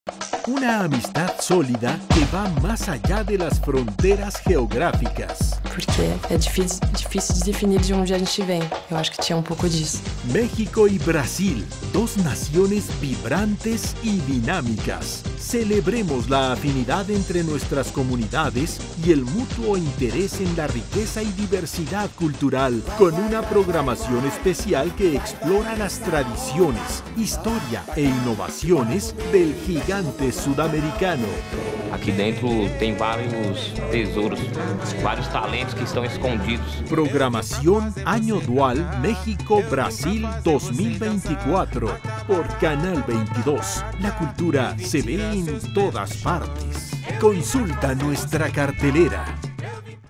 Programación Año Dual México - Brasil 2024 | Promo
Como parte de las actividades del Año Dual México-Brasil 2024, Canal 22 presenta una programación especial con documentales, conciertos, reportajes y más que explora la cultura del país sudamericano

X: @ Canal22 |

 / canal22
Facebook: Canal 22 México |

 / canal22oficial